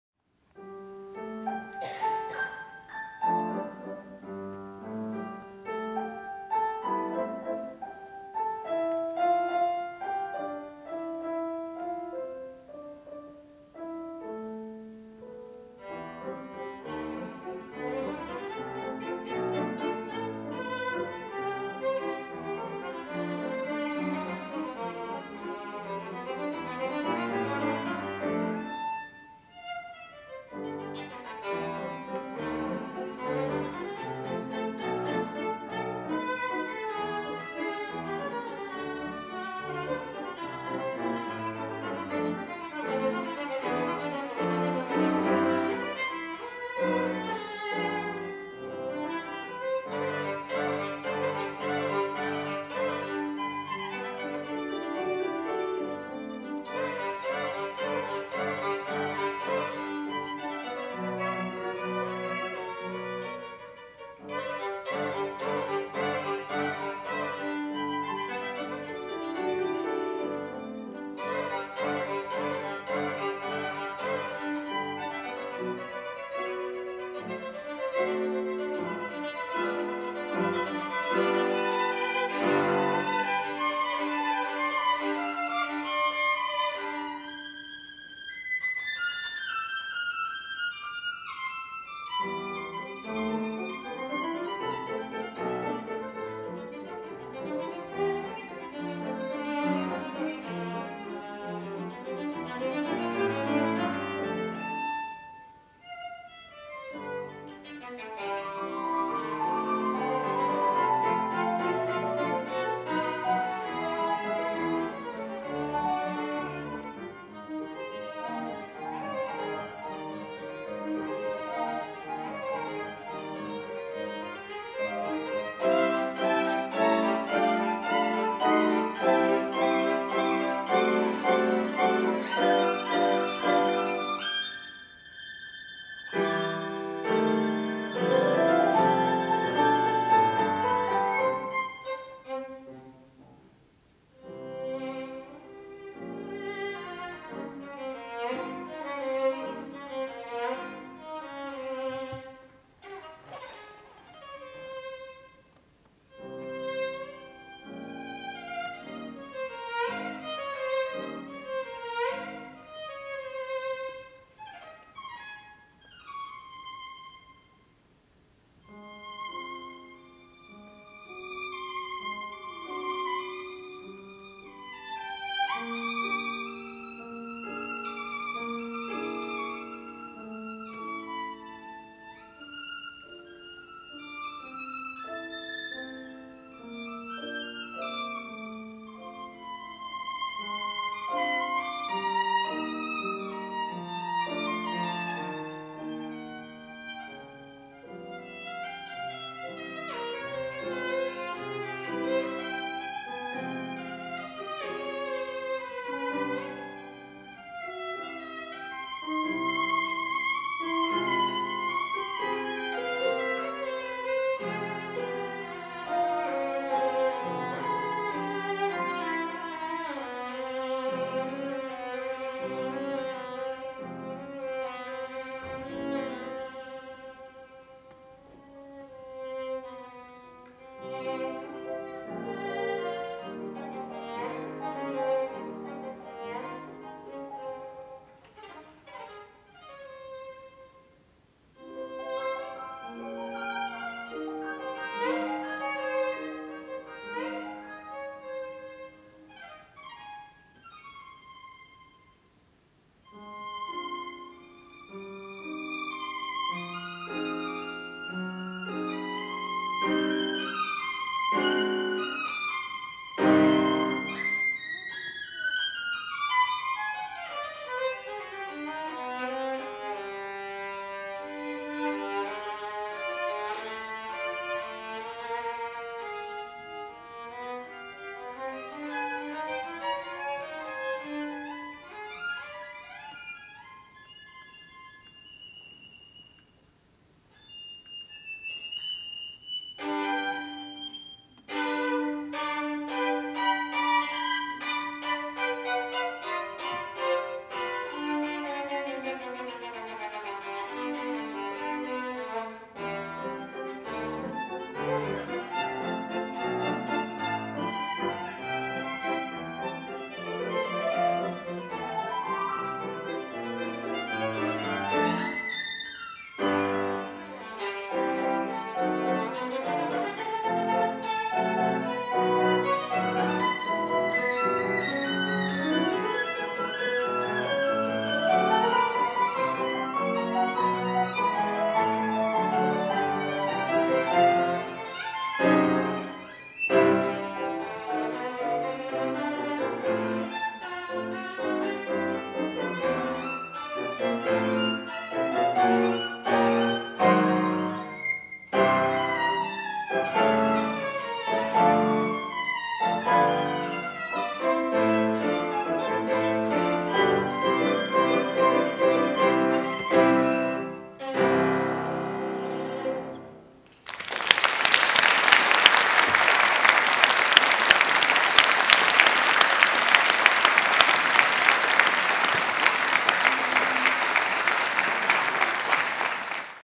ПЕНЗАКОНЦЕРТ - Концерт камерной музыки
Исполнители:
Хироко Нинагава (скрипка, Япония)
Алексей Стариков (фортепиано,Москва)

Большой зал, ГАУК ПО "Пензаконцерт"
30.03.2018